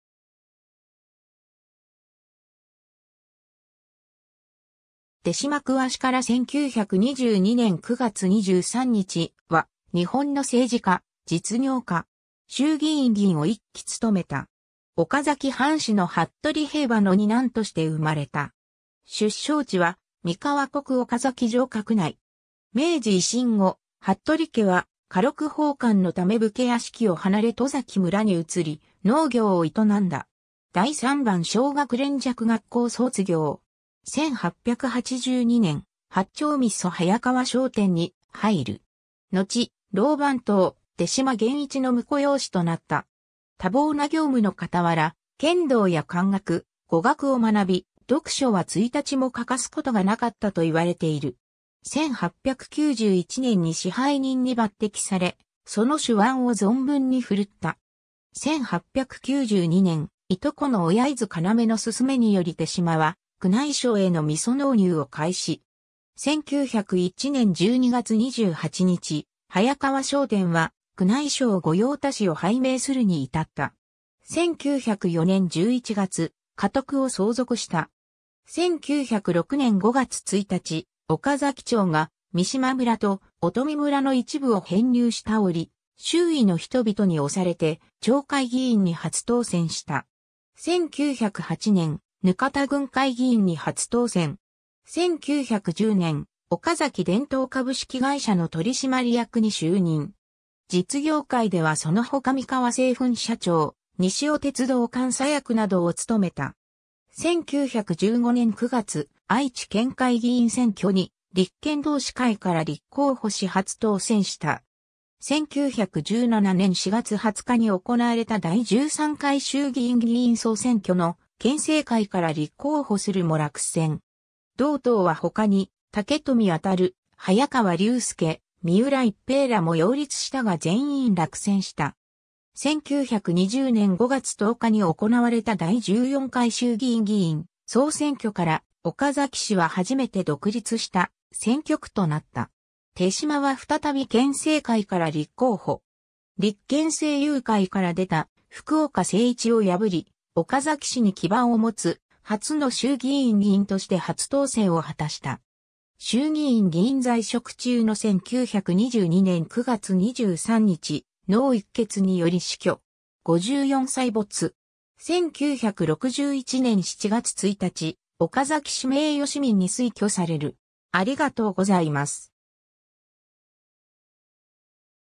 手島鍬司
愛知県選出の衆議院議員_(帝国議会)
手島 鍬司（てしま くわじ、慶応3年9月27日（1867年10月24日） - 1922年9月23日）は、日本の政治家、実業家。
衆議院議員を1期務めた。
 岡崎藩士の服部兵馬の2男として生まれた。
出生地は三河国岡崎城郭内（明治初頭に岡崎康生町となる。現在の岡崎市）。
明治維新後、服部家は家禄奉還のため武家屋敷を離れ戸崎村に移り、農業を営んだ。
第三番小学連尺学校（現・岡崎市立連尺小学校）卒業。
1882年（明治15年）、八丁味噌早川商店（現・カクキュー）に入る。
のち老番頭手島源市の婿養子となった。
 多忙な業務のかたわら、剣道や漢学、語学を学び、読書は一日も欠かすことがなかったと言われている。
1891年（明治24年）に支配人に抜擢され、その手腕を存分にふるった。
1892年（明治25年）、いとこの小柳津要人の勧めにより手島は宮内省への味噌納入を開始。
1901年（明治34年）12月28日、早川商店は宮内省御用達を拝命するに至った。
 1904年（明治37年）11月、家督を相続した。
 1906年（明治39年）5月1日、岡崎町が三島村と乙見村の一部（大字稲熊、大字小呂）を編入した折、周囲の人々に推されて町会議員に初当選した。
1908年（明治41年）、額田郡会議員に初当選。
 1910年（明治43年）、岡崎電灯株式会社の取締役に就任。
実業界ではそのほか三河製粉社長、西尾鉄道監査役などを務めた。
 1915年（大正4年）9月、愛知県会議員選挙に立憲同志会から立候補し初当選した。
 1917年（大正6年）4月20日に行われた第13回衆議院議員総選挙の憲政会から立候補するも落選。
同党は他に武富済、早川龍介、三浦逸平らも擁立したが全員落選した。
 1920年（大正9年）5月10日に行われた第14回衆議院議員総選挙から岡崎市は初めて独立した選挙区（第3区）となった。
手島は再び憲政会から立候補。
立憲政友会から出た福岡精一を破り、岡崎市に基盤を持つ初の衆議院議員として初当選を果たした。
 衆議院議員在職中の1922年（大正11年）9月23日、脳溢血により死去。
54歳没。
 1961年（昭和36年）7月1日、岡崎市名誉市民に推挙される。